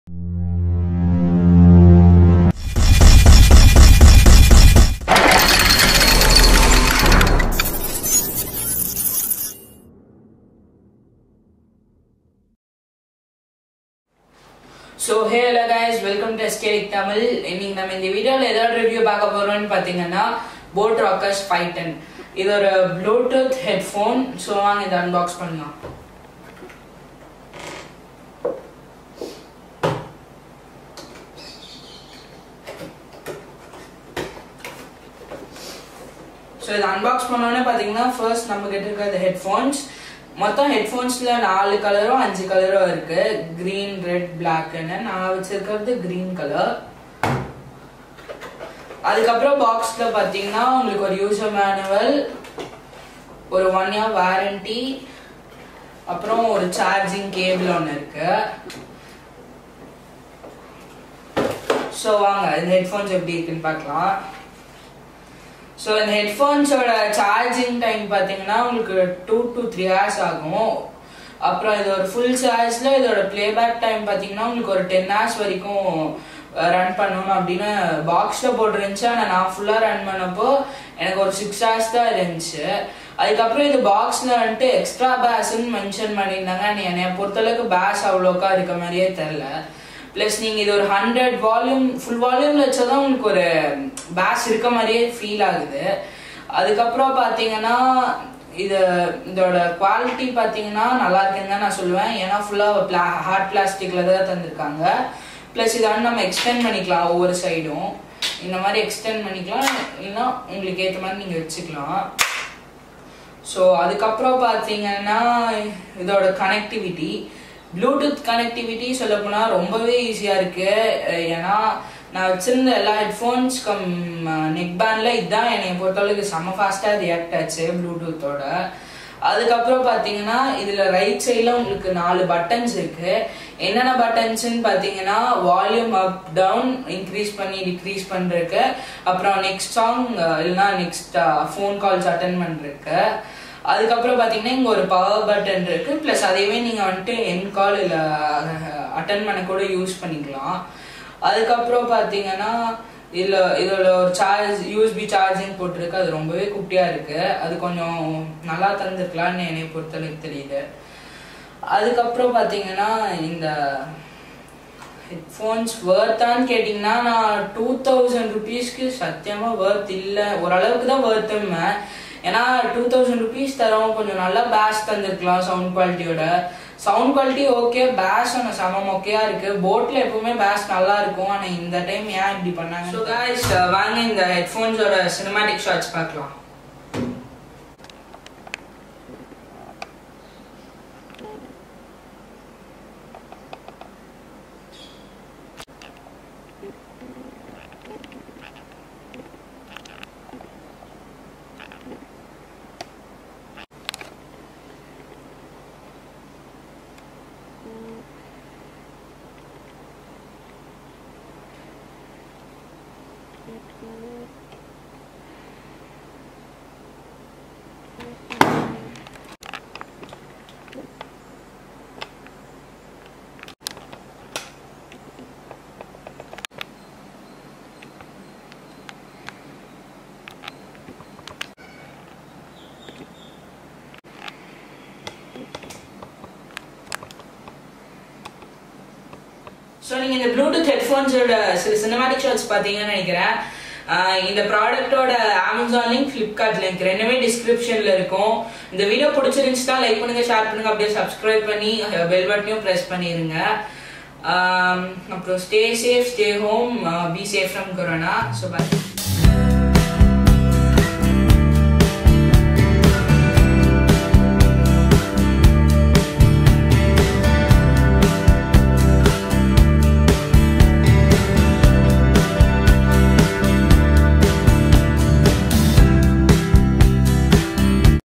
Boat Rockerz 510 | Review and Unboxing | Best One !! | Sanjay Krishna
In this video I am reviewing one of the best Bluetooth headphone .